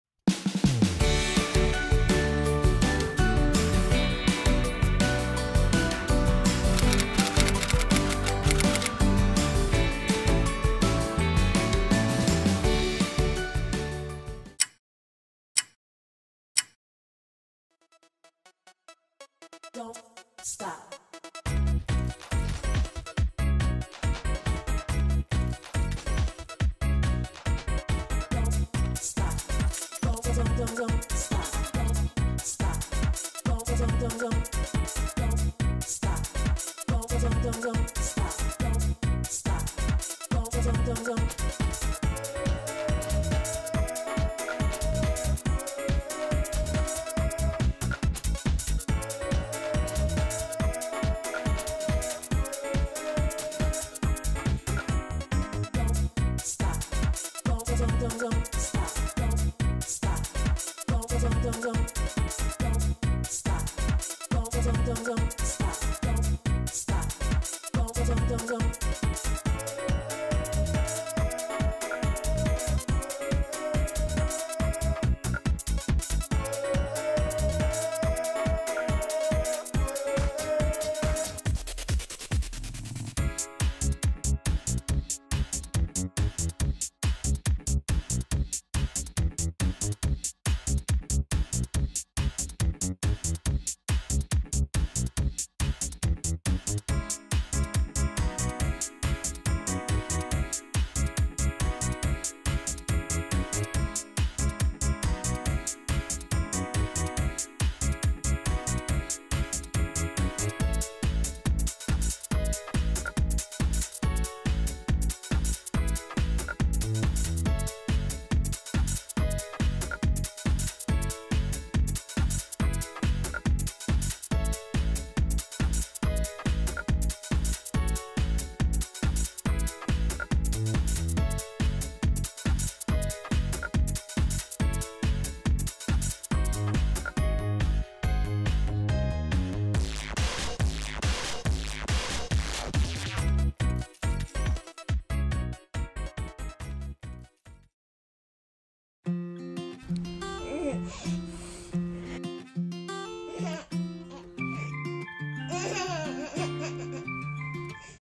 【#107●8倍速】デザートのときは自分で食べます！9-month-old baby can eat by herself only when eating the fruit!
『0歳9ヶ月の赤ちゃんハナがひたすら美味しそうに離乳食を食べる動画』
“9-month-old baby girl Hana happily eats her solid food:)"

♡Hana's items♡

ライスシリアル | rice cereal

ベビーチェア | baby table&chair